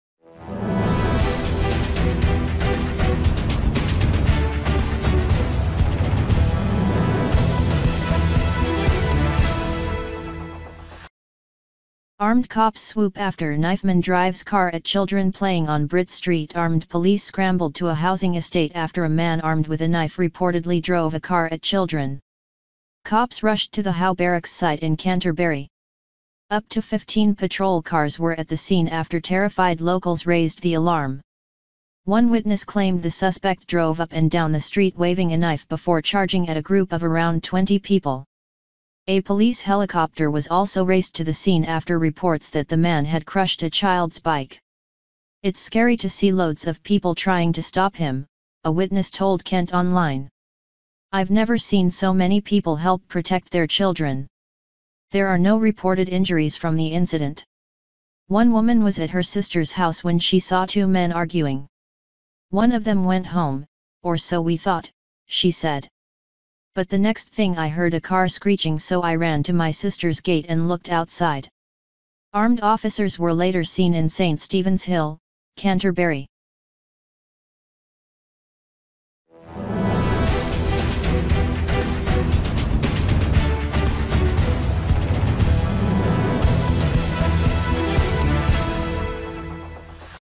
Armed cops swoop after 'knifeman drives car at children playing on Brit street'
ARMED police scrambled to a housing estate after a man armed with a knife reportedly drove a car at children.

Cops rushed to the Howe Barracks site in Canterbury.

Up to 15 patrol cars were at the scene after terrified locals raised the alarm.